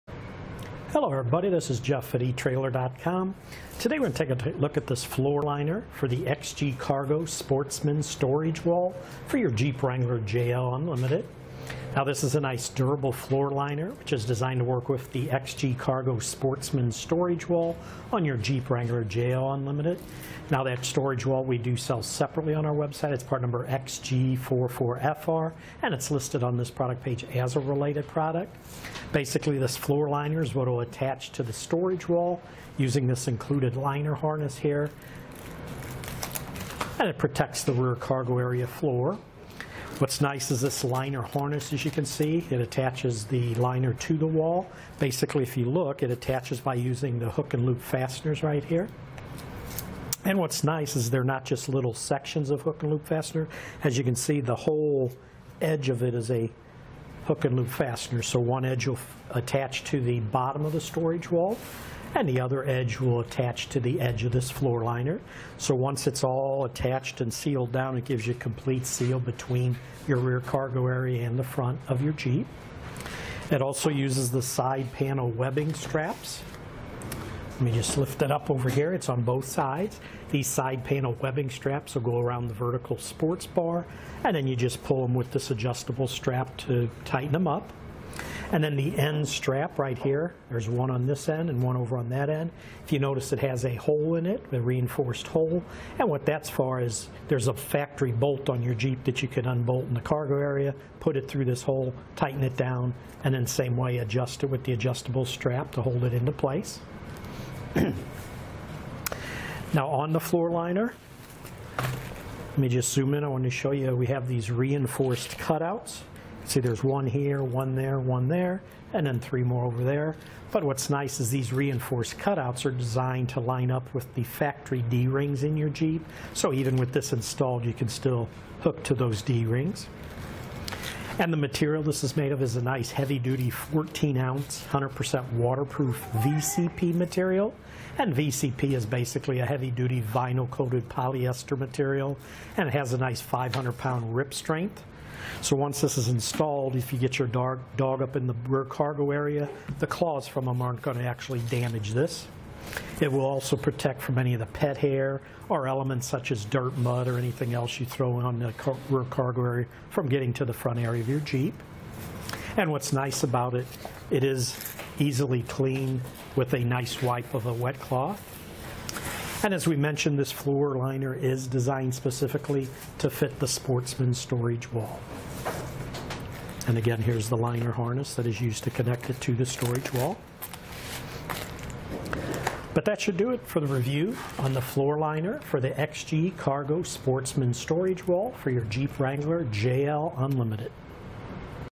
etrailer | Review of XG Cargo Vehicle Organizer - Storage Wall Floor Liner - XC47FR
Click for more info and reviews of this XG Cargo Car Organizer,Jeep Storage:

Hello everybody, this is Jeff at etrailer.com. Today, we're gonna take a look at this floor liner for the XG Cargo Sportsmen Storage Wall for your Jeep Wrangler JL Unlimited. Now, this is a nice durable floor liner, which is designed to work with the XG Cargo Sportsmen Storage Wall, on your Jeep Wrangler JL Unlimited. Now that storage wall, we do sell separately on our website, it's part number XG44FR. And it's listed on this product page as a related product. Basically this floor liners what'll attache to the storage wall, using this included liner harness, here.

And it protects the rear cargo area floor. What's nice is this liner harness, as you can see, it attaches the liner to the wall. Basically, if you look, it attaches by using the hook and loop fasteners, right here. And what's nice is they're not just little sections of hook and loop fastener. As you can see, the whole edge of it is a hook and loop fastener.

So, one edge will attach to the bottom of the storage wall, and the other edge will attach to the edge of this floor liner. So, once it's all attached and sealed down, it gives you a complete seal between your rear cargo area and the front of your Jeep. It also uses the side panel, webbing straps. Let me just lift it up over here. It's on both sides.

These side panel, webbing straps will go around the vertical sports bar. And then you just pull them with this adjustable strap to tighten them up. And then the end strap, right here. There's one on this end, and one over on that end. If you notice it has a hole in it, the reinforced hole.

And what that's for is there's a factory bolt on your Jeep that you could unbolt in the cargo area, put it through this hole, tighten it down, and then same way, adjust it with the adjustable strap, to hold it into place. Now, on the floor liner, let me just zoom in. I want to show you that we have these reinforced cutouts. See there's one here, one there, one there, and then three more over there. But what's nice is these reinforced cutouts are designed to line up with the factory D-rings in your Jeep. So even with this installed, you can still hook to those D-rings. And the material this is made of, is a nice heavy duty 14 ounce, 100% waterproof VCP material. And VCP is basically a heavy duty vinyl coated polyester material. And it has a nice 500 pound rip strength. So once this is installed, if you get your dog up in the rear cargo area, the claws from them on actually damage this. It will also protect from any of the pet hair, or elements such as dirt, mud, or anything else you throw on, in the rear cargo area, from getting to the front area of your Jeep. And what's nice about it, it is easily cleaned with a nice wipe of a wet cloth. And as we mentioned, this floor liner is designed specifically to fit the sportsman storage wall. And again, here's the liner harness that is used to connect it to the storage wall. But that should do it for the review on the floor liner for the XG Cargo Sportsmen Storage Roll for your Jeep Wrangler JL Unlimited..